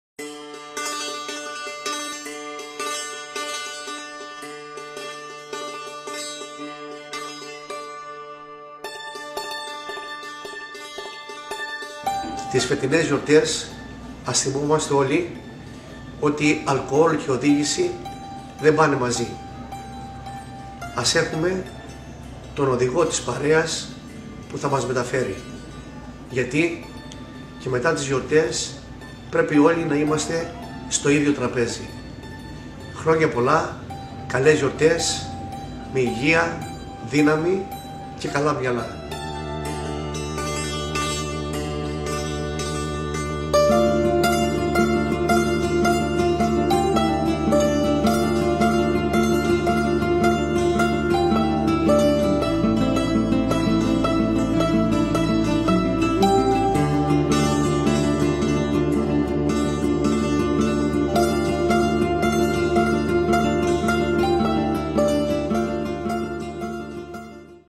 Σταύρος Αρναουτάκης - Καμπάνια Κόκκινου Κρήτης 88,4 fm
Σταύρος Αρναουτάκης - Περιφερειάρχης Κρήτης
Αλκοόλ και οδήγηση στις γιορτές
Καμπάνια του Κόκκινου Κρήτης 88,4 fm και inotos.gr
Μουσική: Κωνσταντής Παπακωνσταντίνου
Ηράκλειο Κρήτης